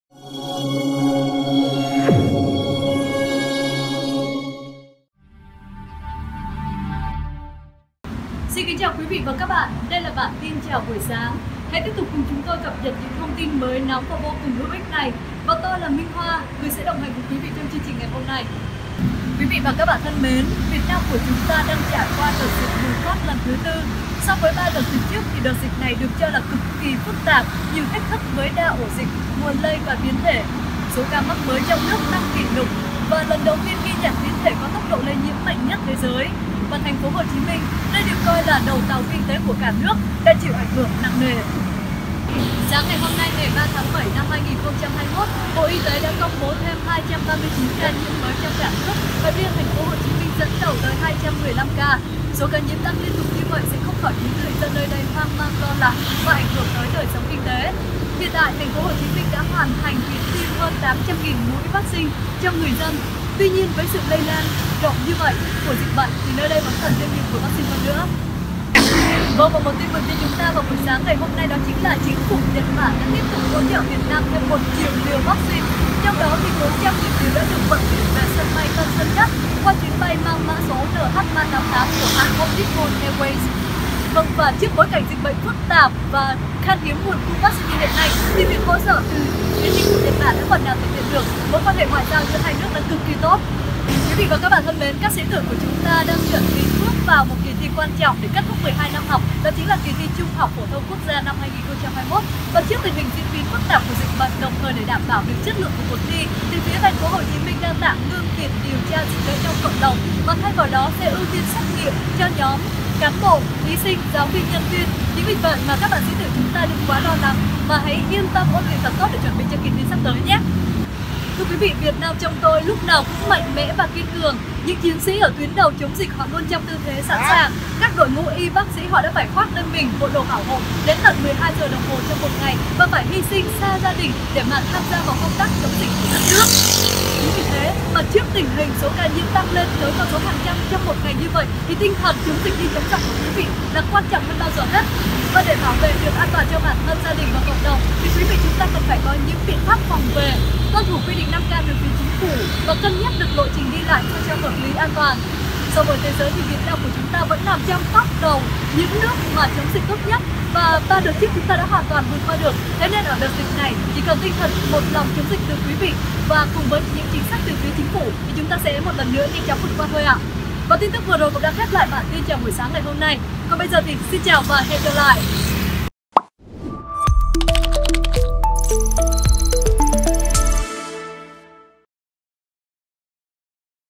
Bài dự thi: "BẢN TIN CHÀO BUỔI SÁNG" - Thí sinh Phạm Thị Minh Hoa
Việt Nam trong tôi lúc nào cũng đoàn kết và kiên cường. Những chiến sĩ trong tuyến đầu chống dịch Covid luôn trong tư thế sẵn sàng, các đội ngũ y bác sĩ họ đã phải khoác lên mình bộ đồ bảo hộ liên tục 12 tiếng trong một ngày và phải hy sinh xa gia đình để tham gia vào công tác chống dịch bệnh của đất nước.Thế nên, trong thời điểm dịch bệnh căng thẳng lên tới vài trăm ca nhiễm mới mỗi ngày như hiện nay, tinh thần “ chống dịch như chống giặc “ ở mỗi chúng ta lại càng quan trọng hơn bao giờ hết. Để đảm bảo an toàn cho bản thân, gia đình và cộng đồng,  chúng ta phải luôn có những biện pháp bảo vệ, tuân thủ quy định 5K từ chính phủ, cân nhắc lộ trình đi lại sao cho hợp lý an toàn.

So với thế giới, thì Việt Nam đang nằm trong top đầu những nước chống dịch tốt nhất, chúng ta hoàn toàn thành công ở 3 đợt dịch trước, chính vì vậy mà ở đợt dịch này, chỉ cần tinh thần một lòng chống dịch từ người dân cùng với những chính sách từ chính phủ, ta sẽ một lần nữa vượt qua nhanh chóng."
Sản phẩm dự thi của bạn Phạm Thị Minh Hoa - Thí sinh cuộc thi tìm kiếm MC - BTV tài năng 2021, chủ đề "Việt Nam trong tôi".
Mọi người hãy cùng nhau bình chọn và cỗ vũ bạn ấy nhé.